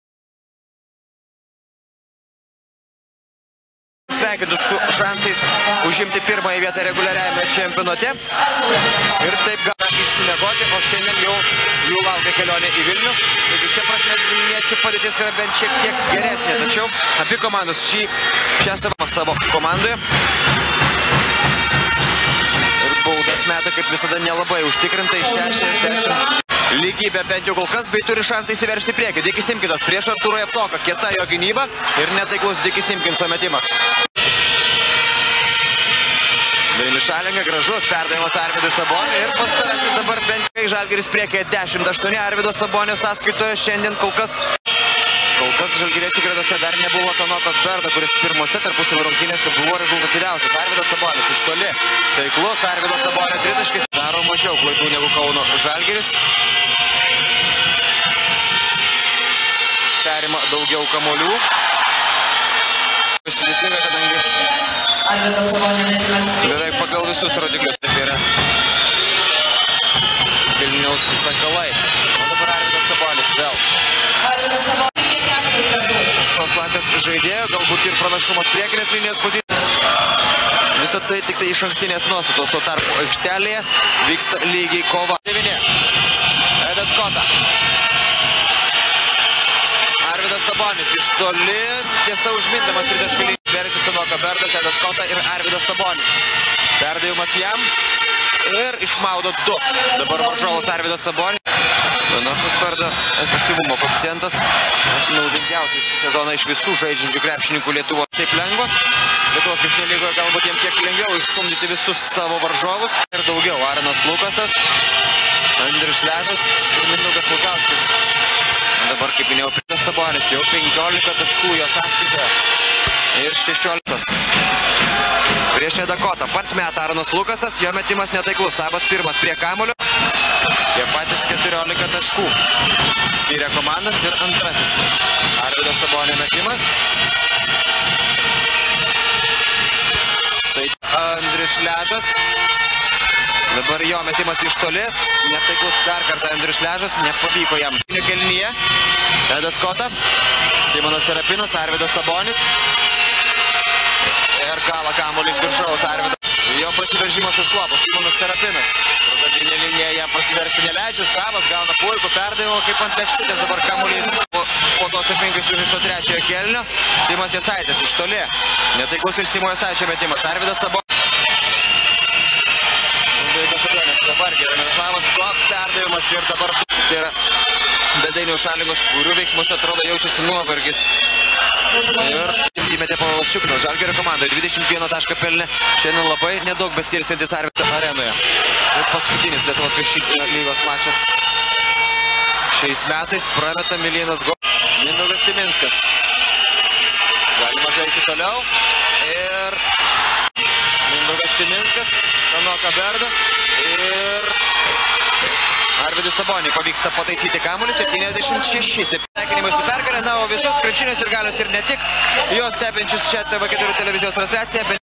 Sabonis vs Lietuvos Rytas (2004)
In his last year, Arvydas Sabonis plays agaisnt Lietuvos Rytas in the  Lithuanian National Basketball League in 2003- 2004 season.
Sabonis still has it: long shoot, pass view, hook-shots, rebounds… Stats: 23 points, 10 rebounds, 3 assists.

En su último año en activo, Arvydas sabonis juega en la Liga Nacional de Lithuania contra el Lietuvos Rytas en la temporada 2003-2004.
Sabonis todavía tiene el "toque", con tiros de larga distancia, visión de juego, ganchos, rebotes... Estadísticas: 23 puntos, 10 rebotes, 3 asistencias.